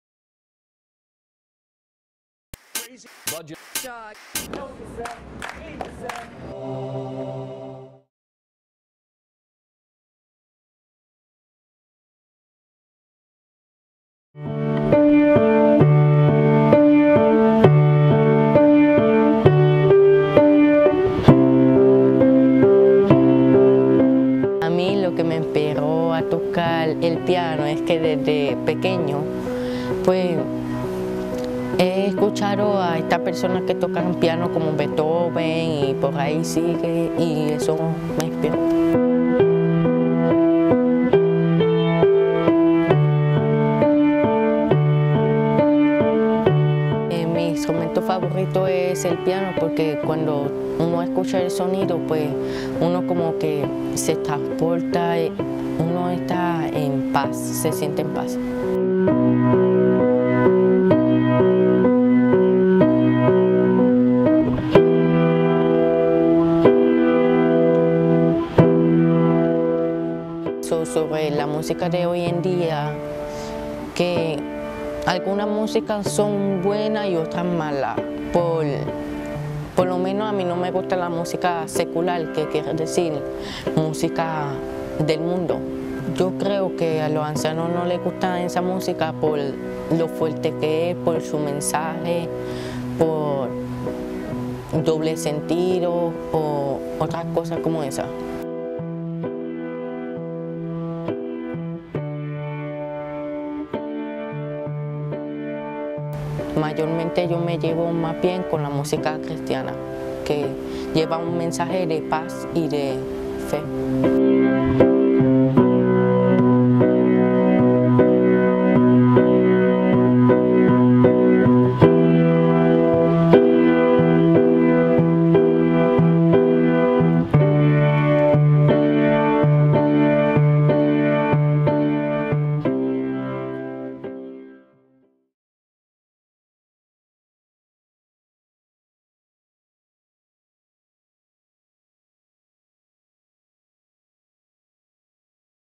Play My Life
A micro-documentary about the importance of music to one young man.  Written and produced by Karina Maldonado during Press Pass TV's first Media Leadership Institute in Holyoke, MA.